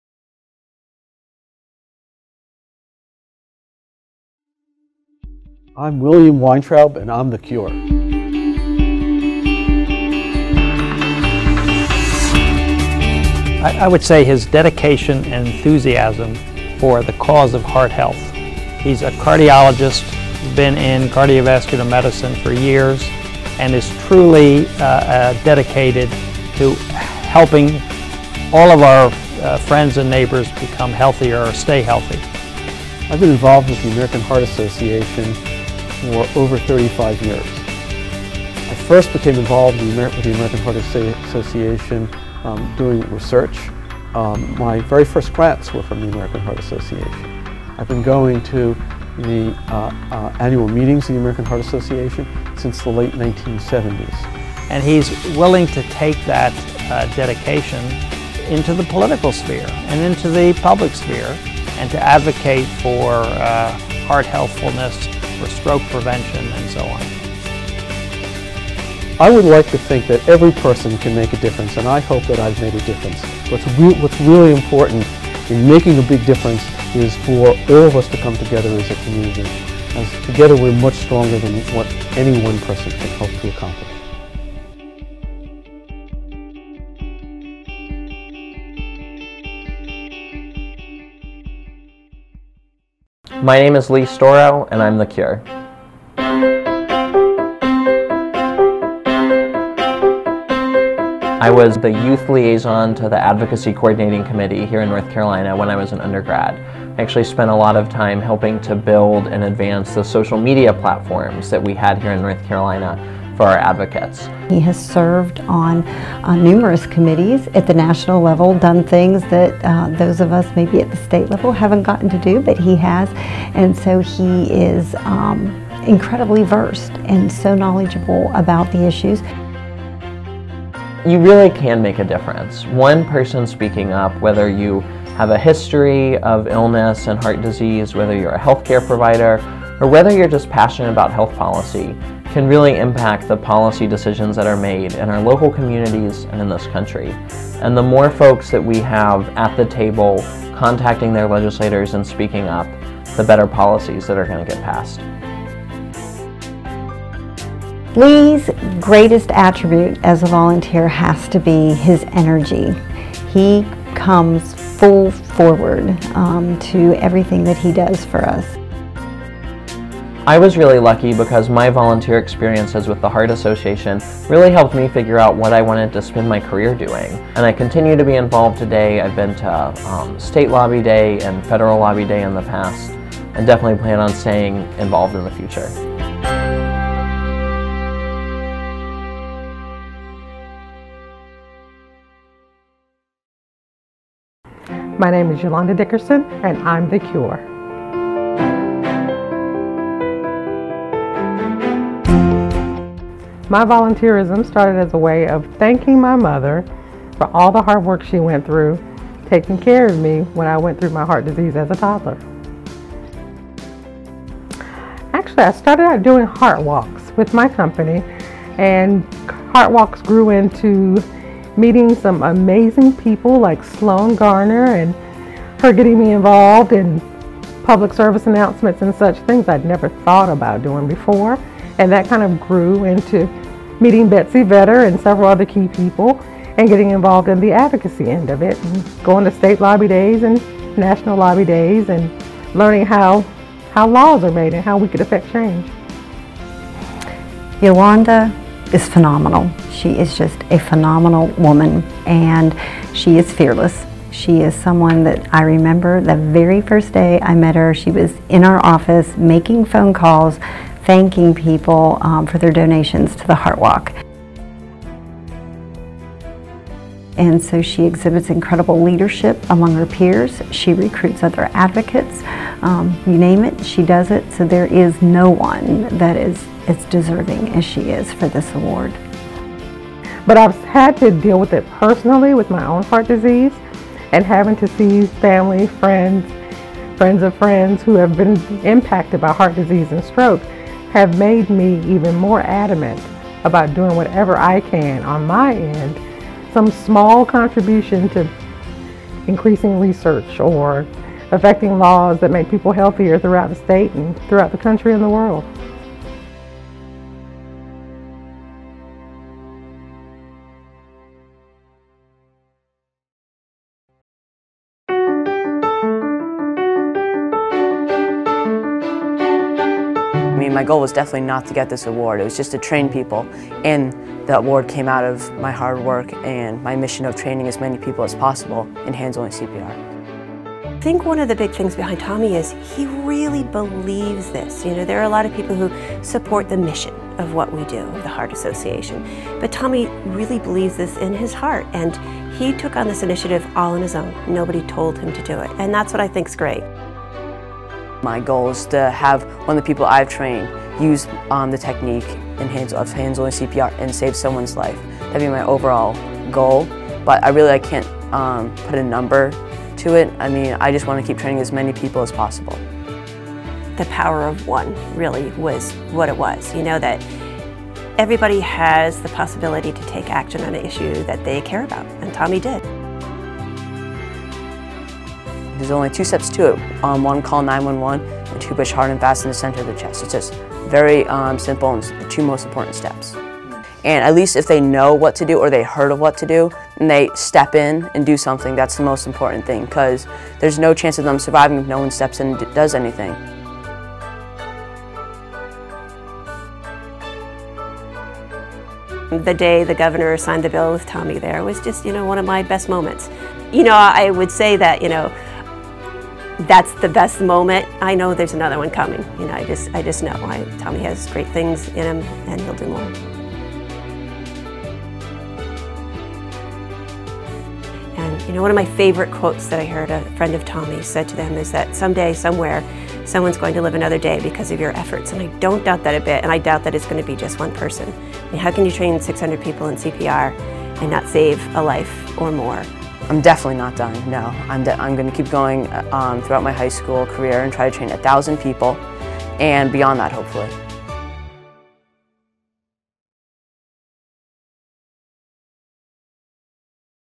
American Heart Association Honors 2013 Advocates of the Year
Congratulations to the American Heart Association's 2013 Advocates of the Year!  These outstanding volunteers have demonstrated a firm commitment to the mission of the association and taken action to advance public policy priorities at the local, state, and national levels to build healthier communities and healthier lives, free of heart disease and stroke.